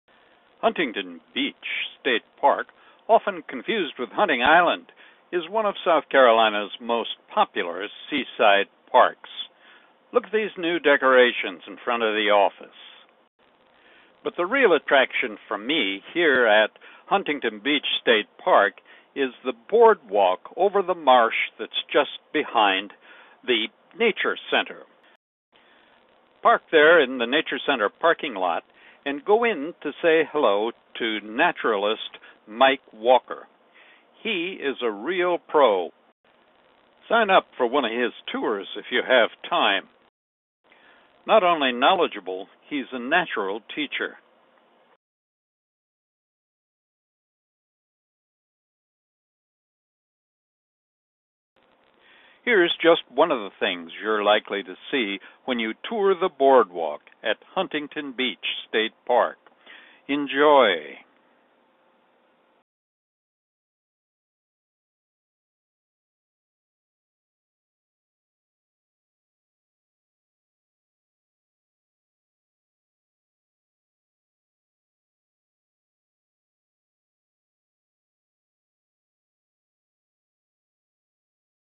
Marsh Boardwalk at Huntington Beach State Park South Carolina
Everyone has their favorite feature at Huntington Beach State Park. The Huntington's Atalaya Mansion gets lots of votes. The beautiful Atlantic Ocean beach naturally ranks up there. But for me, the long wooden boardwalk built out over the marsh behind the nature center takes the cake. A remarkable place. Here are a few glimpses of what you'll see when you visit.